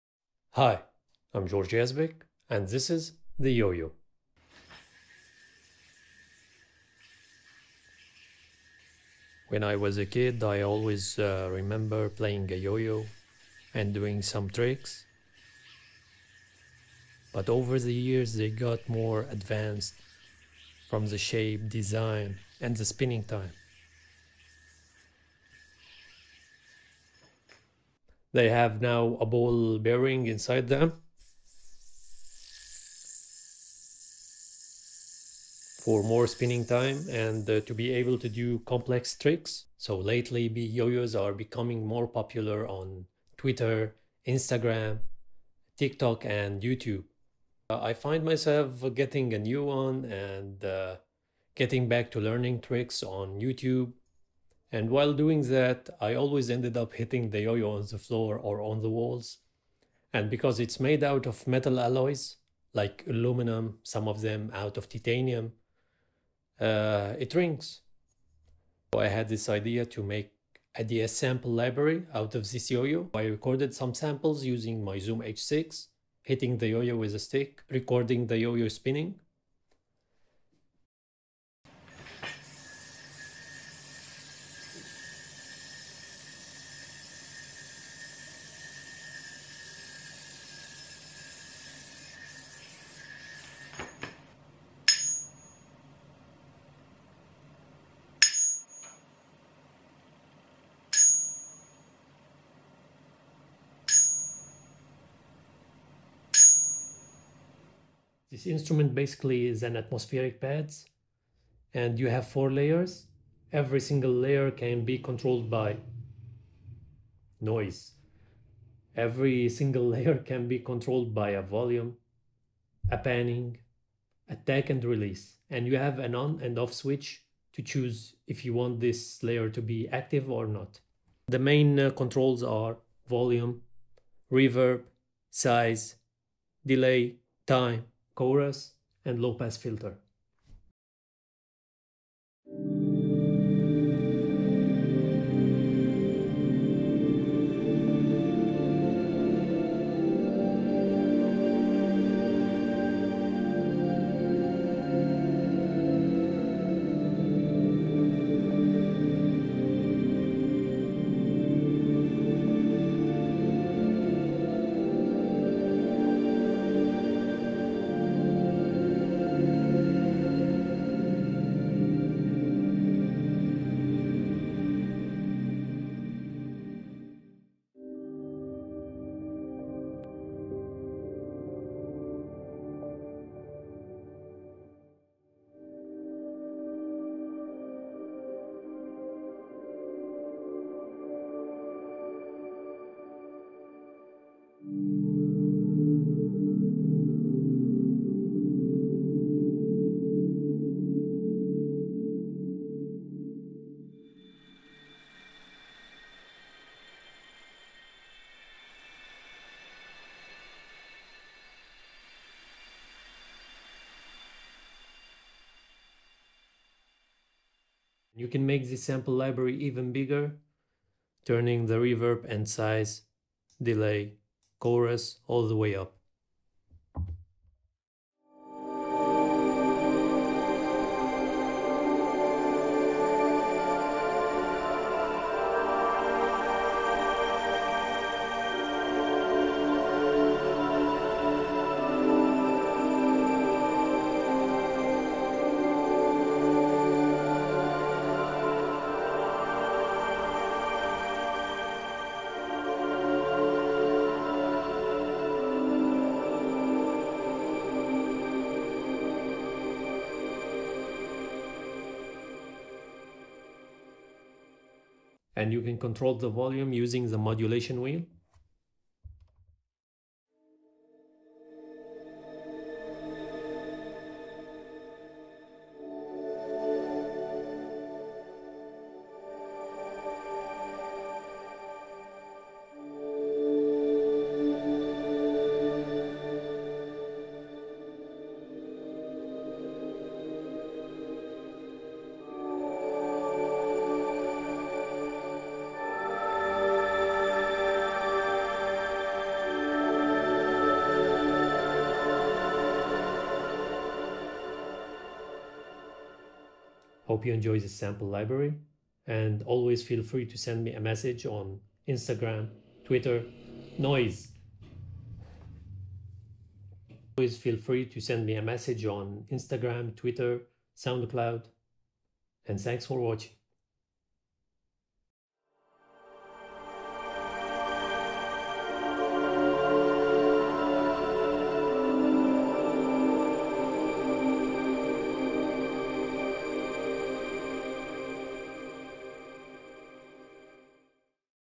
The story and testing (The YoYo sample library pianobook)
George Yazbek - The Yoyo

Yoyos have always been so popular, but lately they've been massively popular on Instagram tiktok and YouTube.
They had lots of new modifications from the desgin, shape and the spinning time

So I got back to yoyoing after a very long time and when I started learning tricks i always ended up hitting the yoyo on the floor or the wall.
and because it's made out of metal alloys like aluminium or titanium it rings.

So I recorded samples using my zoom h6
Hitting the yoyo with a stick, recorded the yoyo spinning and from those samples I made this massive atmospheric decent sampler sample library

The layout:

You have 4 layers and every single layer can be controlled by:
1. On/Off switch
2. Volume
3. Panning
4. Attack
5. Release

Main controls:
1. Volume (you can control it with the mod wheel)
2. Reverb
3. Size
4. Delay
5. Time
6. Chorus
7. And low pass filter

I would be so thankful to hear back from you!
And let me know if your use it to make music.